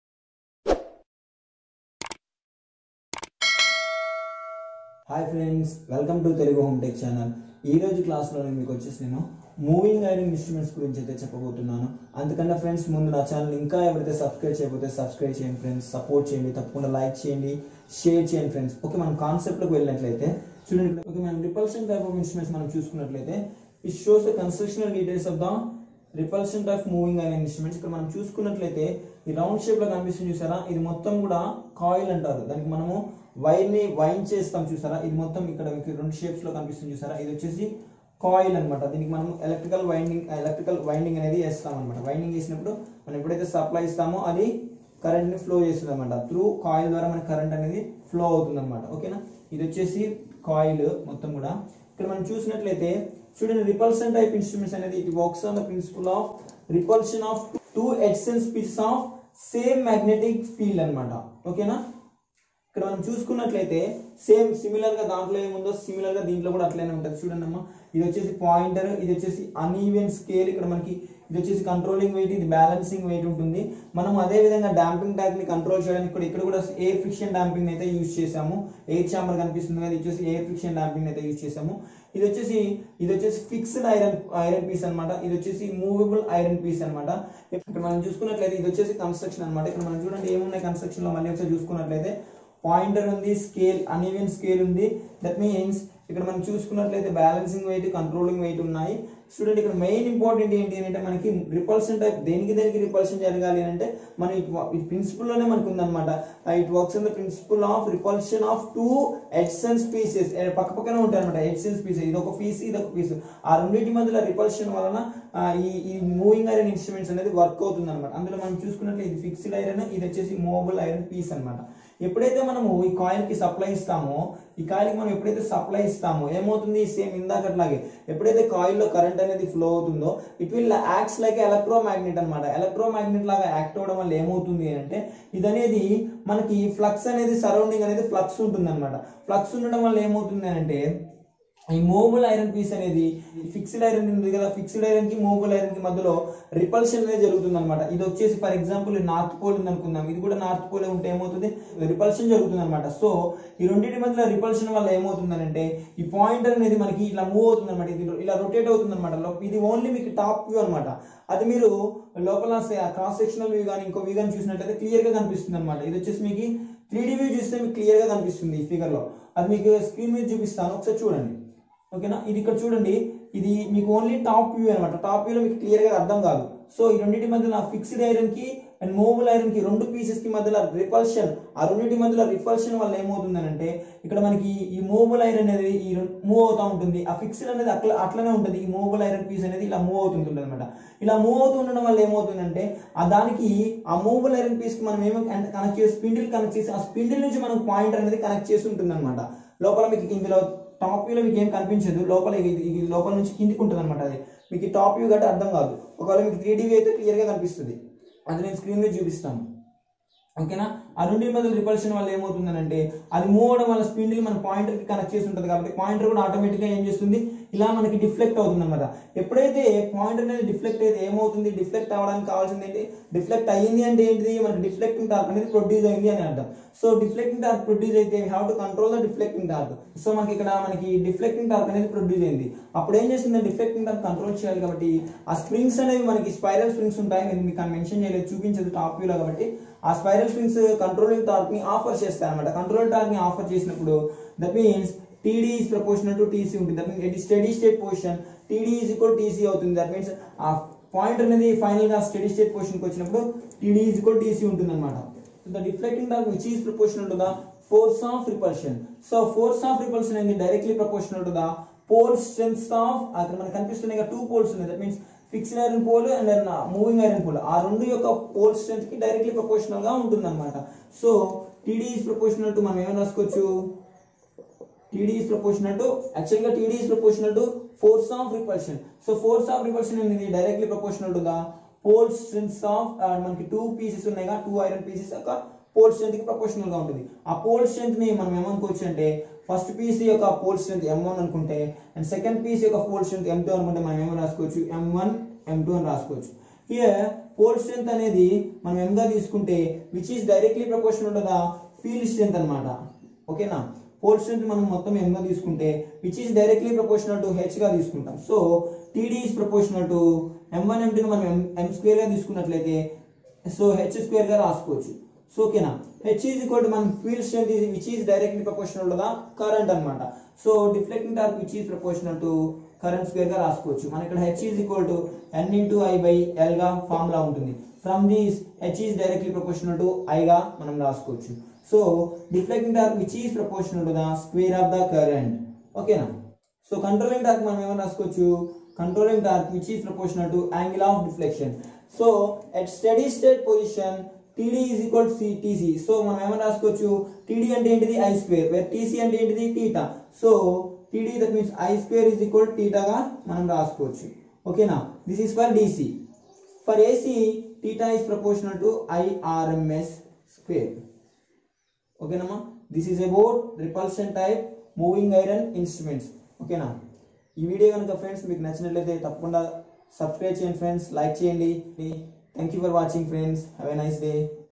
Repulsion_type_moving_iron_instrument in Telugu moving iron instrument in Telugu
Repulsion_type_moving_iron_instrument in Telugu moving iron instruments in Telugu
construction and working of repulsion type instrument.